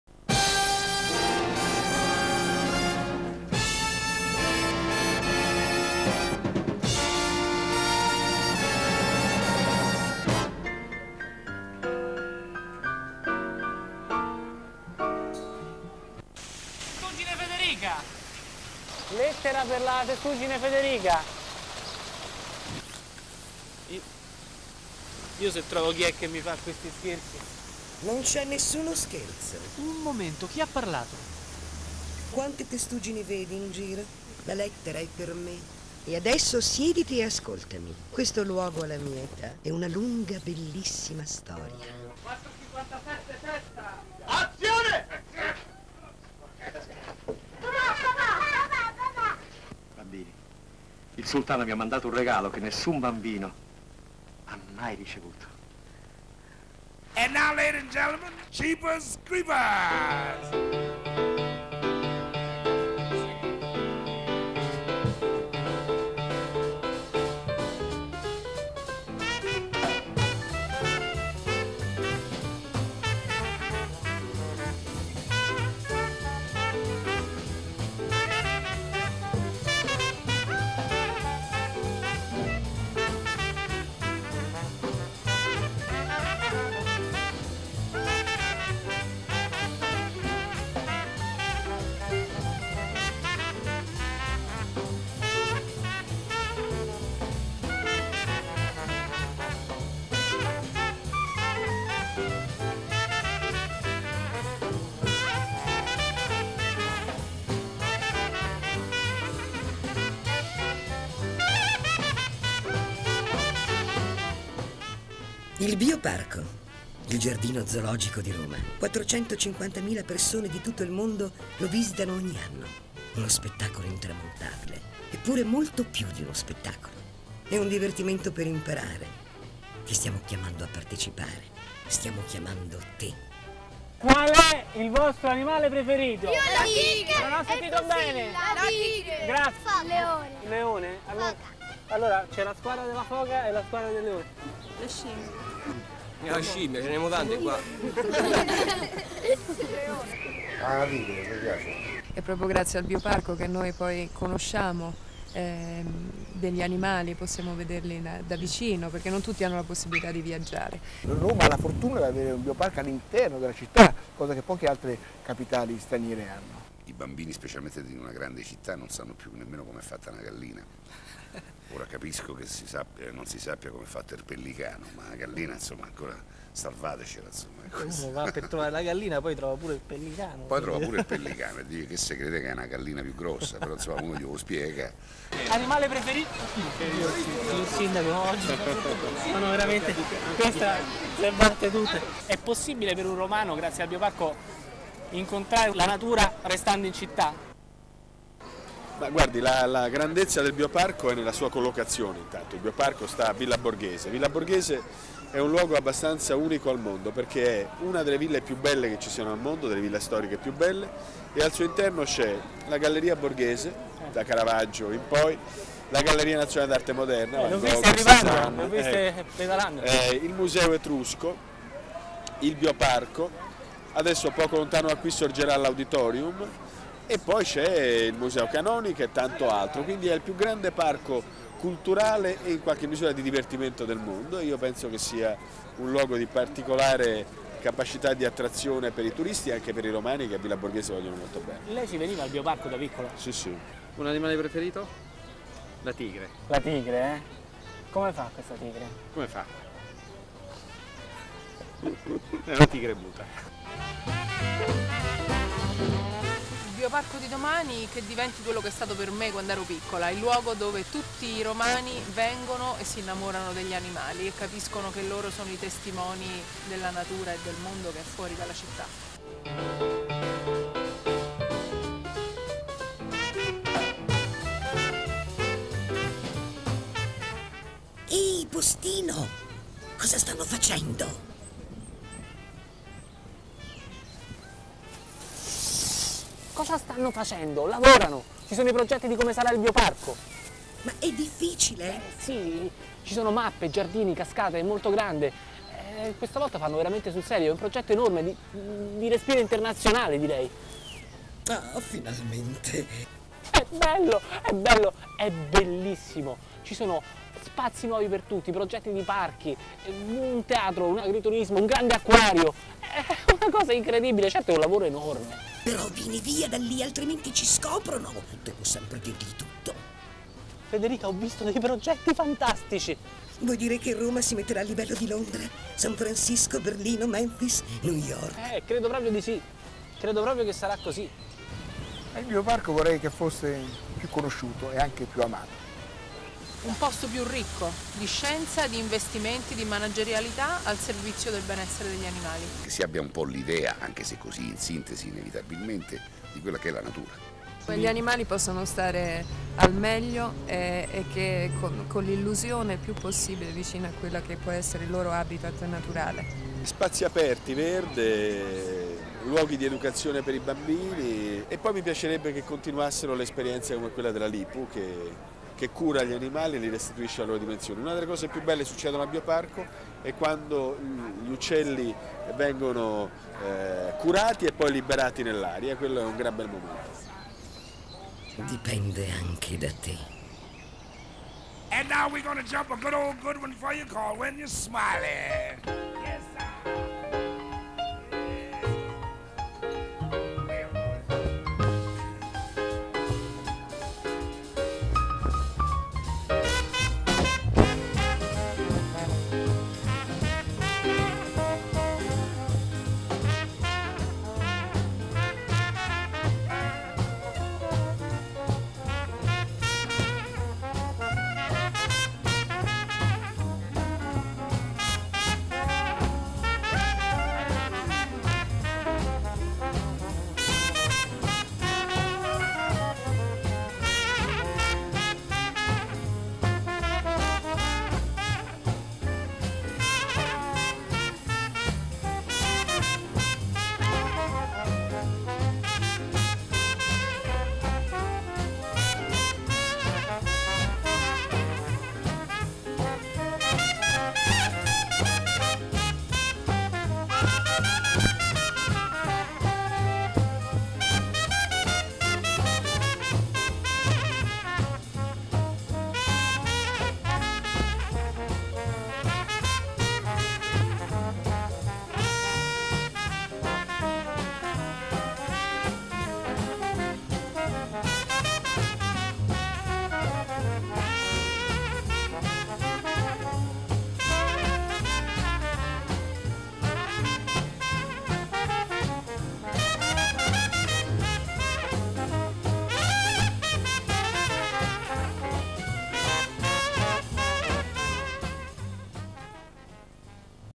Il Parco della Vita
Un video girato con la mia regia, con Valter Veltroni, Gigi Proietti, Claudia Khol, Fulco Pratesi, Monica Cirinnà... Per quei bambini che vivono nele grandi città e dicono "Mamma, guarda... Questa mela sa di shampoo". Di qusto video sono particolarmente fiero delle riprese aeree del colosseo (all'inizio) e di Trinità dei Monti (con un raro movimento di macchina dall'alto, nel finale).